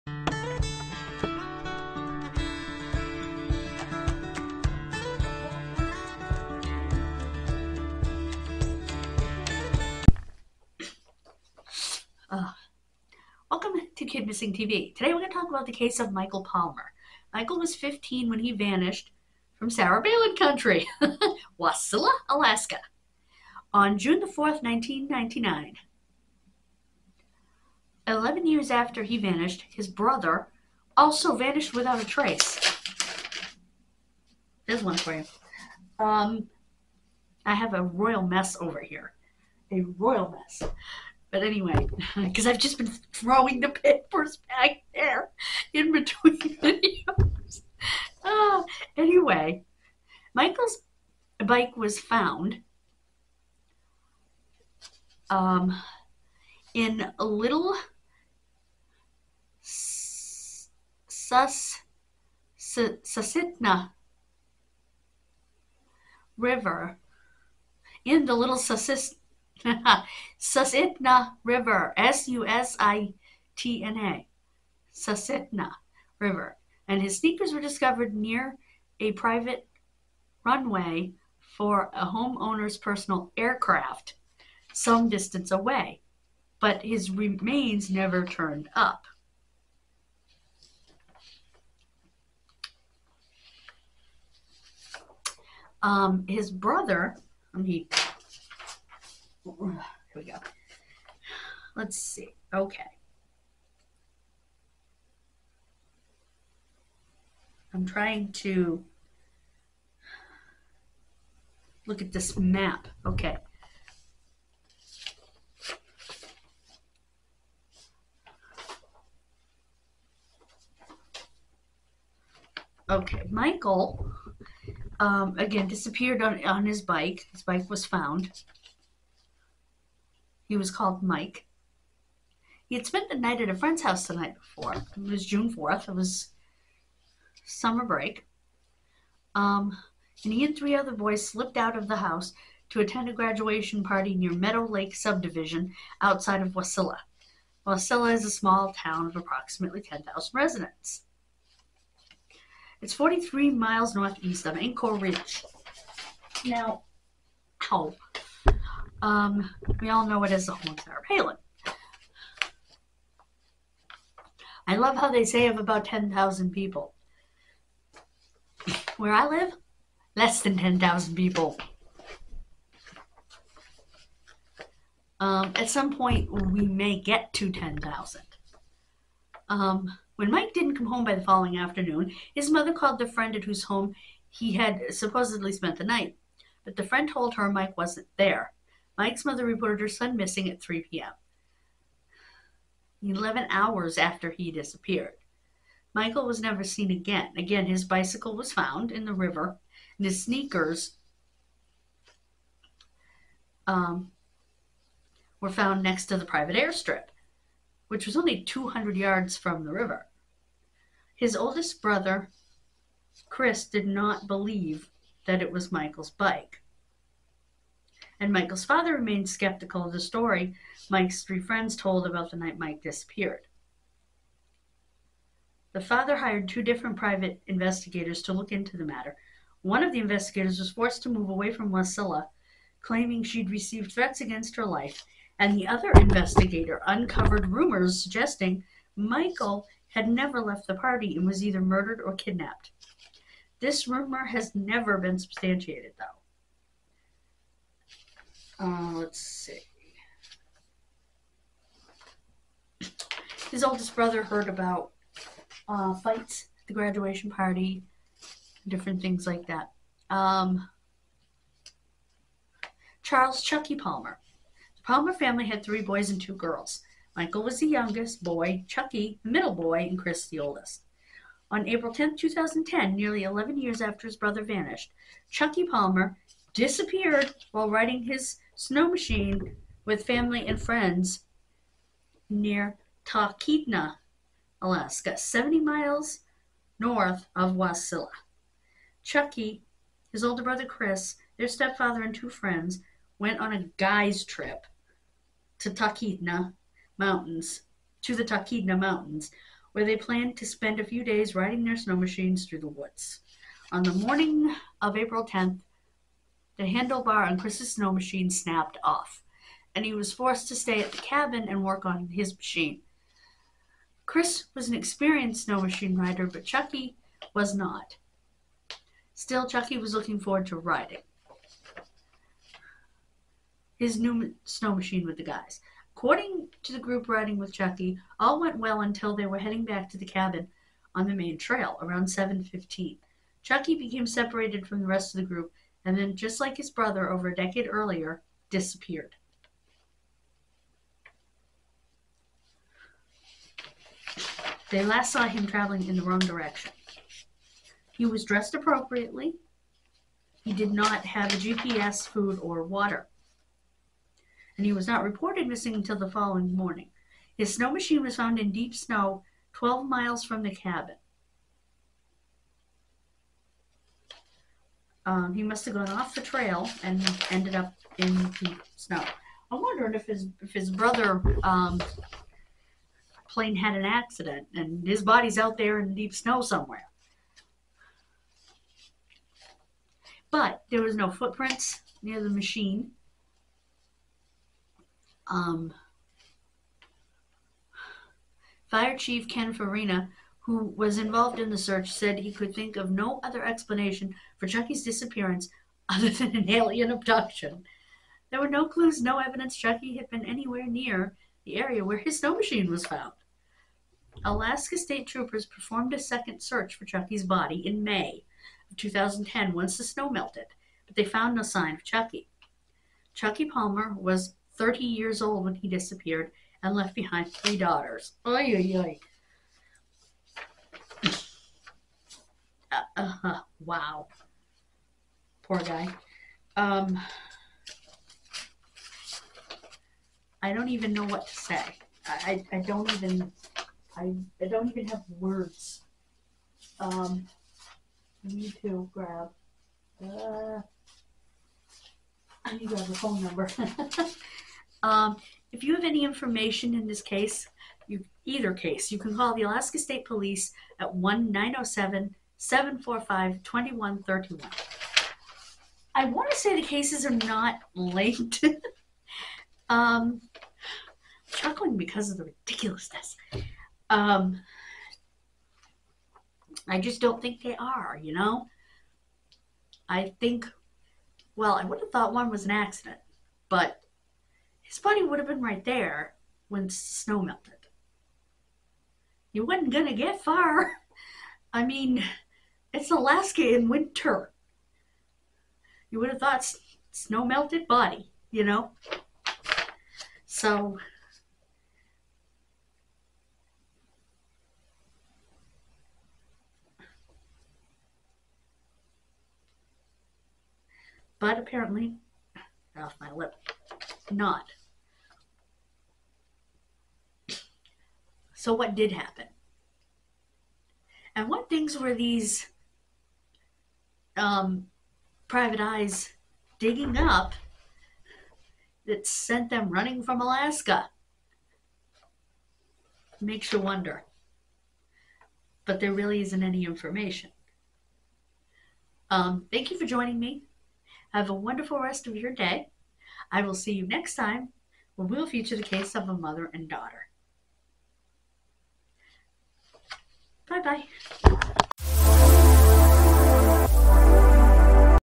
Two Brothers Vanish From Their Small Alaska Hometown Years Apart
Are the cases of  missing brothers Michael and Chucky Palmer cases related?